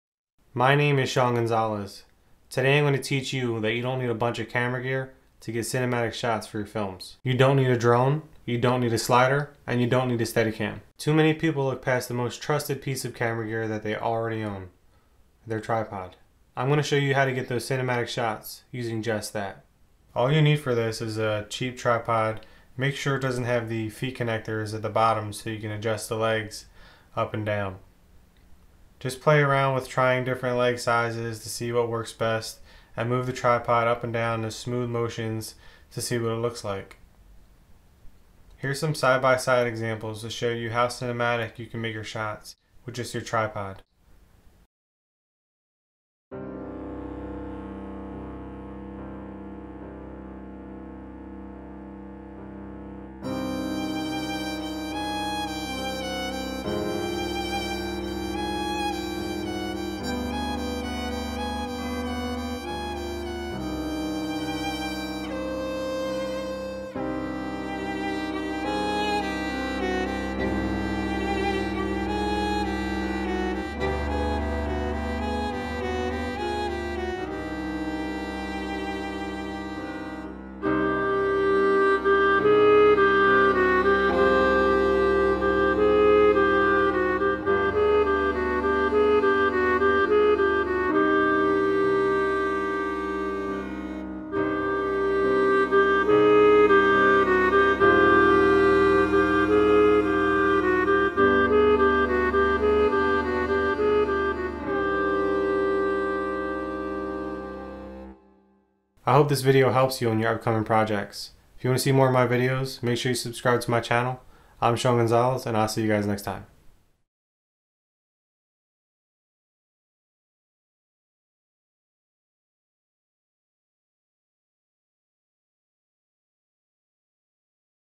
How To Get Cinematic Shots By Using Only Your Tripod HD 2016
This is something I had been wanting to show you for a while.Its an easy way to get cinematic shots for your short or feature films by using just your tripod.Too may people take their tripod for granted and focus on more expensive equipment that could stop you from shooting your project.I hope you learned from the video and dont forget to SUBSCRIBE to my channel for more videos.

Gear:
T2I/550D
Pentax 50mm 1.7
Cinemorph filter
CHEAP ALUMINUM TRIPOD